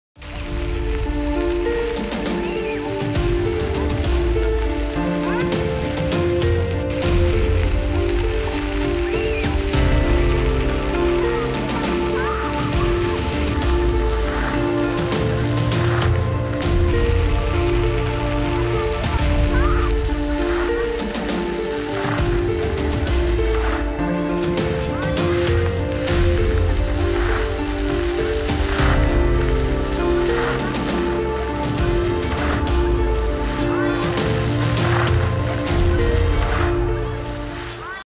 MEYDANA IŞIKLI FISKİYE
Belediyemizin 📍 Demokrasi Meydanında tamamladığı ışıklı fıskiyeler farklı figürleriyle hemşehrilerimiz ve özellikle çocuklar tarafından büyük ilgi görüyor.